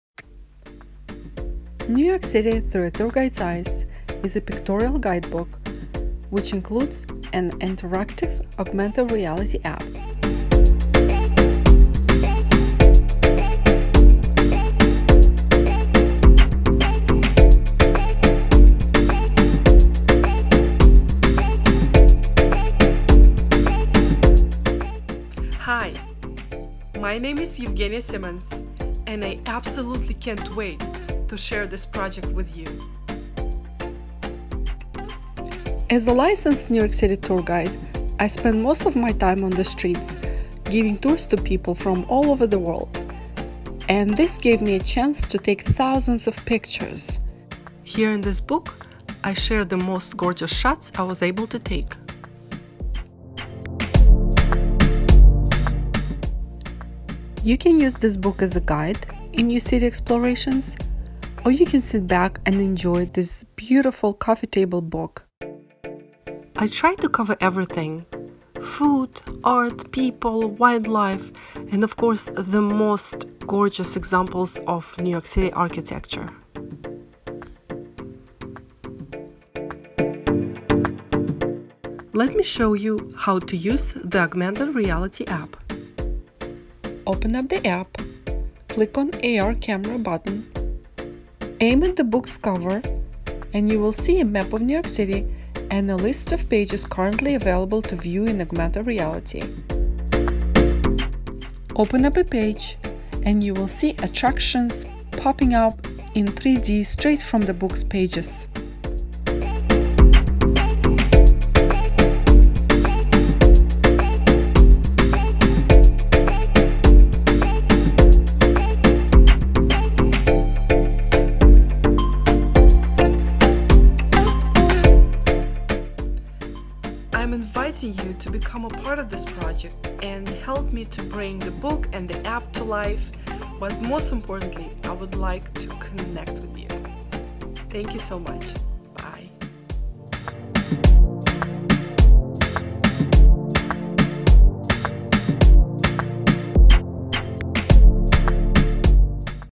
New York City Through a Tour Guide's Eyes.
A photographic guidebook with an interactive augmented reality app.
 Hi! My name is Evgeniya Simmons and I am so excited to share "New York City Through a Tour Guide's Eyes" with you! It is a photographic guidebook with an interactive augmented reality app, designed to spark your interest not only in New York City itself, but in art, architecture, culture, and so much more. The photographs will draw you back time and time again, discovering new details each time you look through the book. The augmented reality app will enhance your experience by bringing virtual models out of the book’s pages into three-dimensional space.

As a licensed New York City tour guide for over six years, I spent most of my time on the streets, giving tours to people from all over the world. This gave me a chance to see the best of New York's landmarks and I was lucky to capture a lot of those gorgeous moments on camera. Here in this beautiful coffee table guidebook I am sharing my favorite moments with you.
BOOK SPECS

Starting Pledge: $1